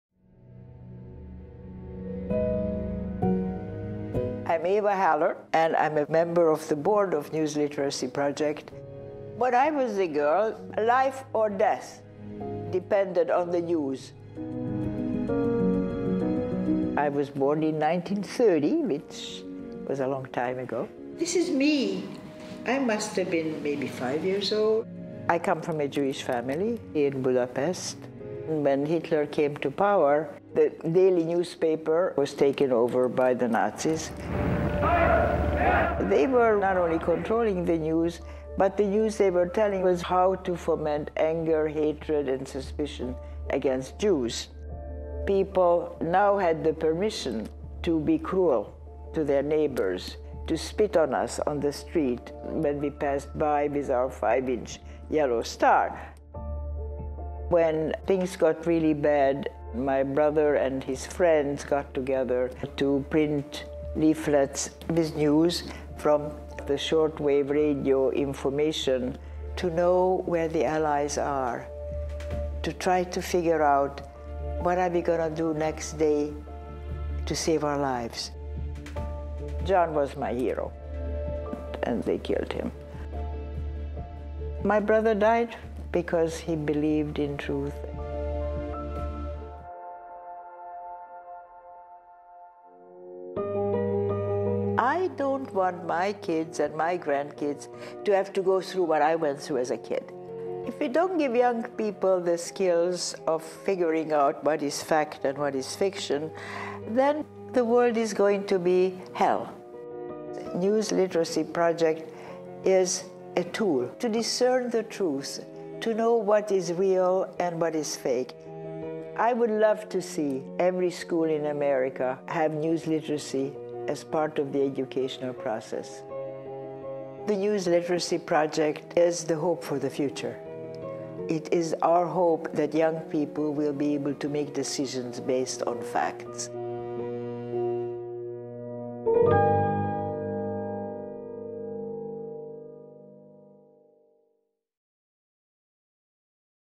‘Life or death depended on the news’
Holocaust survivor Eva Haller personally experienced the brutal consequences of disinformation. For her, news literacy offers hope for the future. (NLP)